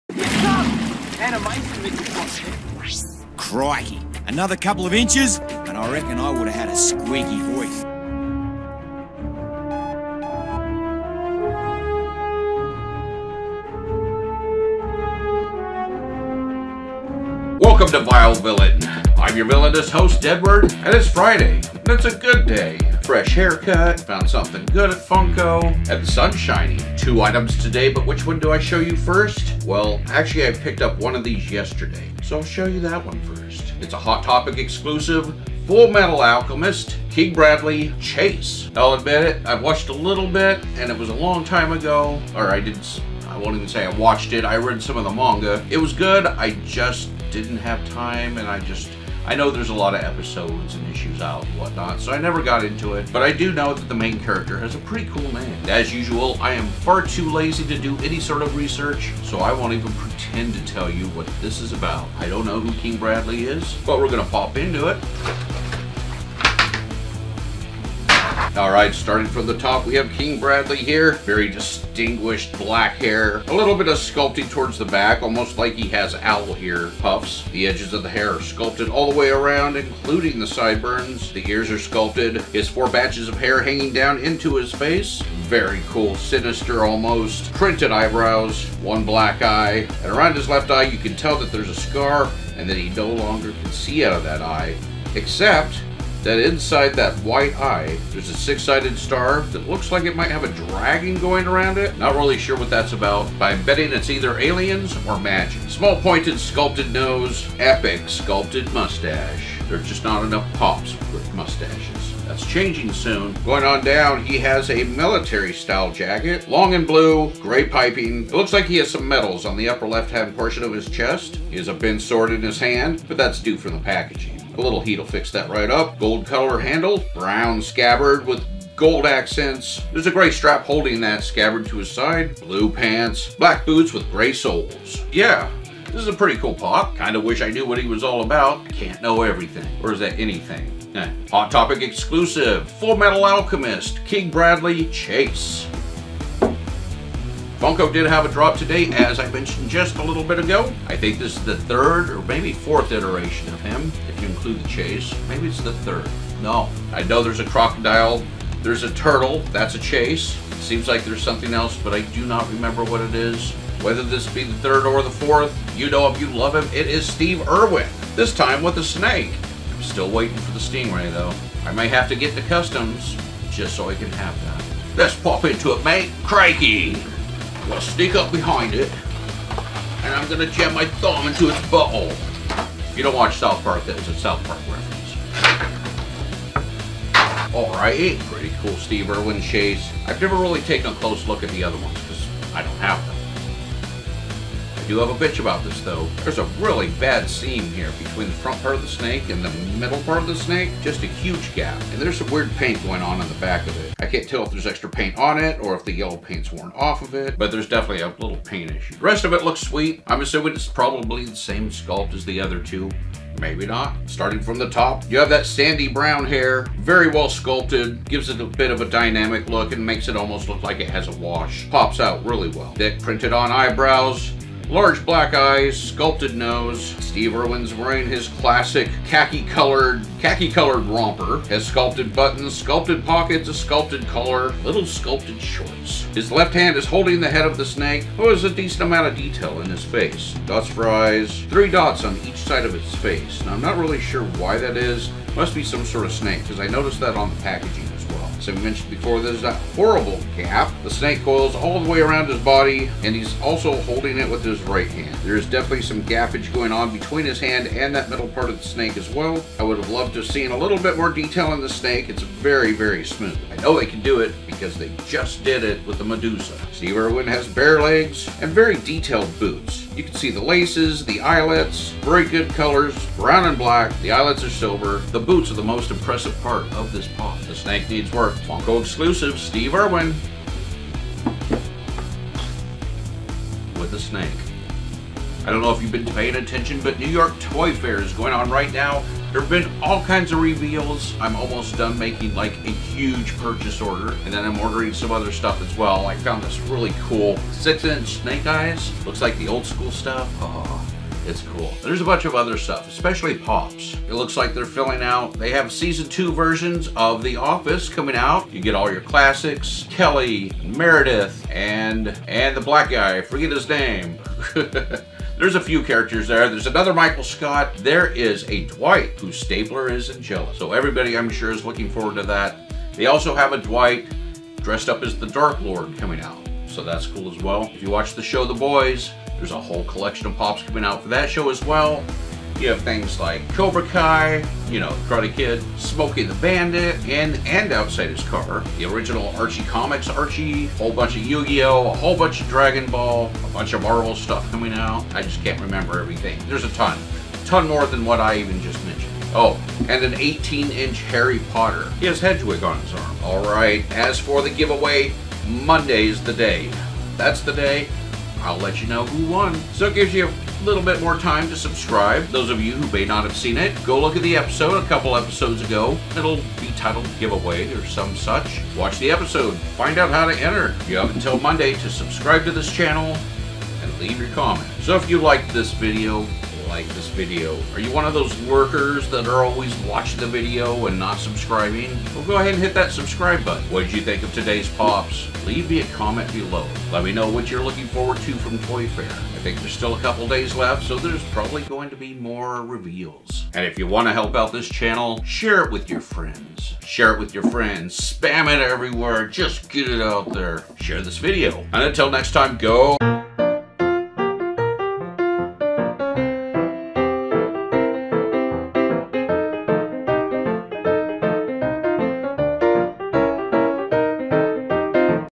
FUNKO EXCLUSIVE: STEVE IRWIN POP! + HOT TOPIC EXCLUSIVE CHASE!
FUNKO EXCLUSIVE: STEVE IRWIN w/ SNAKE POP!
HOT TOPIC EXCLUSIVE: *CHASE* KING BRADLEY POP from FULL METAL ALCHEMIST anime!